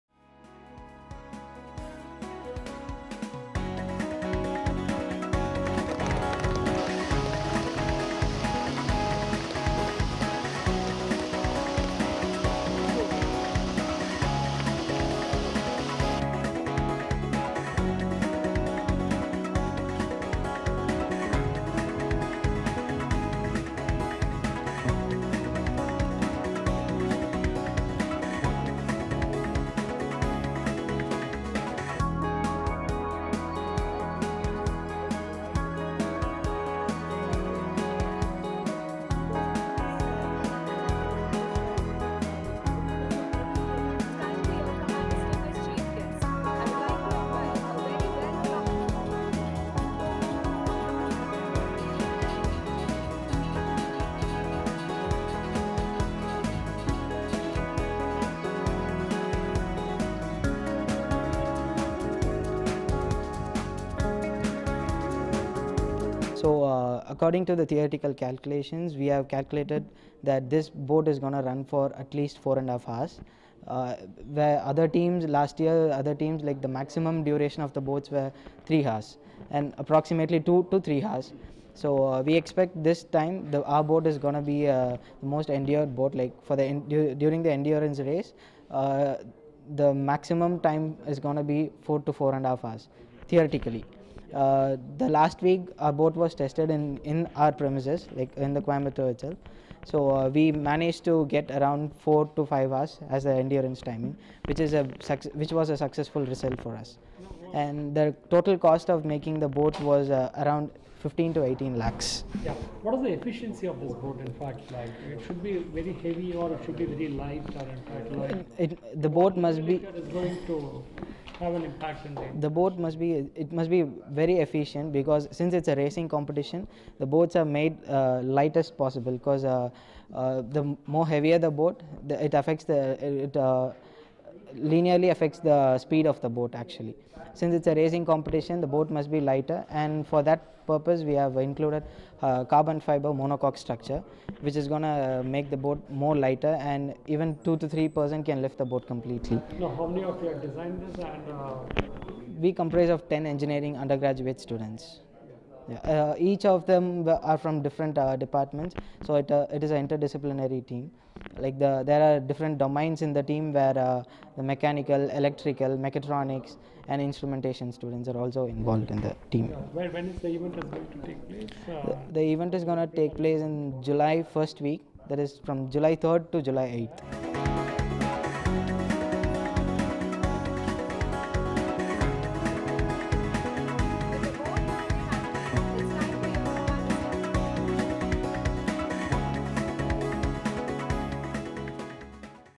Tamil Nadu Students Develop India's First Hydrogen-Powered Energy Boat
A group of 10 engineering students from Kumara Guru College of Engineering in Tamil Nadu's Coimbatore have developed a hydrogen-powered energy boat, which is the first of its kind in India. This is the only team from India which will be taking part in the Monaco Energy Boat challenge to be held in Monaco during the first week of July. The boat and its propulsion were indigenously designed by the students and will compete against international boat racers from across the world.